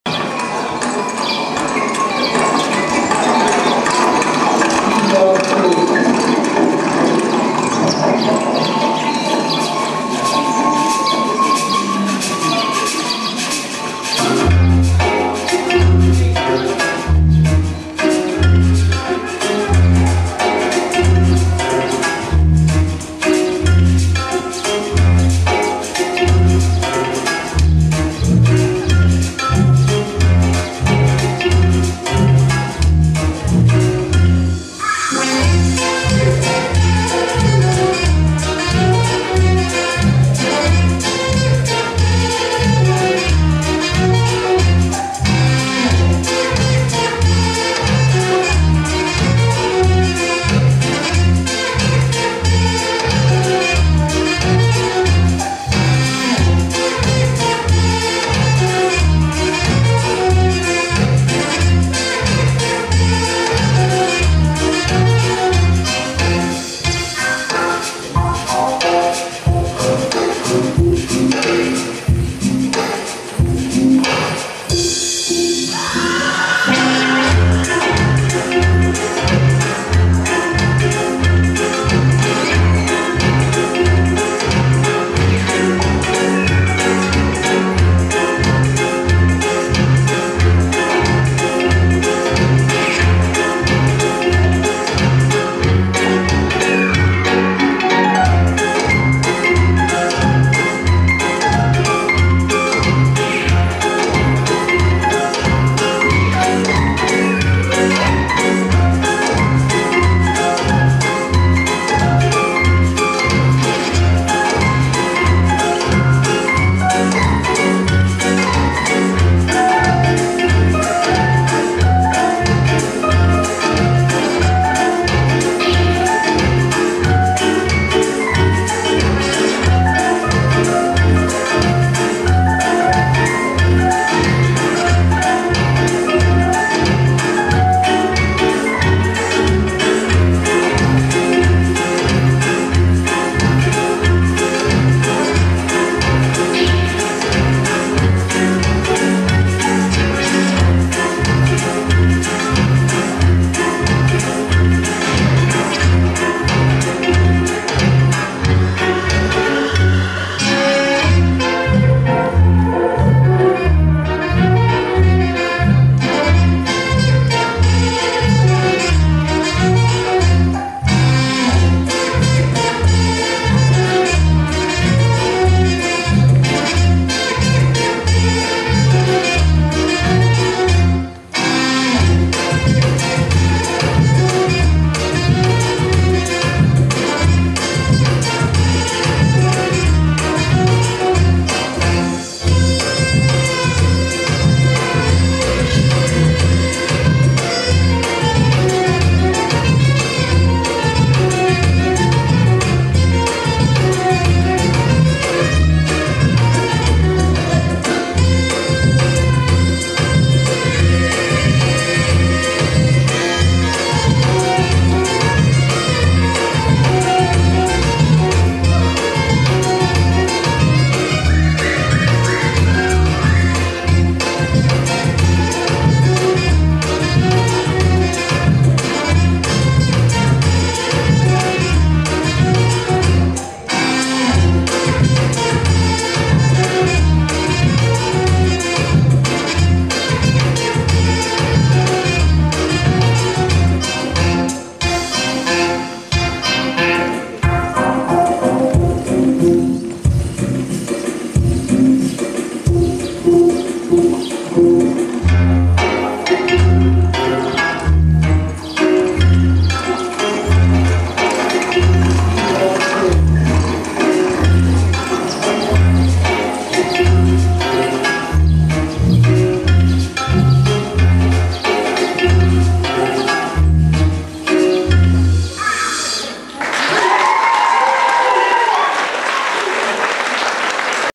BellyTricks Tribal Fusion BVAD July 2011
BellyTricks performing a fun tribal fusion bellydance piece at Blackwater Valley Arabic Dance Hafla July 2011

Choreography by Melissa Hoare
Filming by Smurf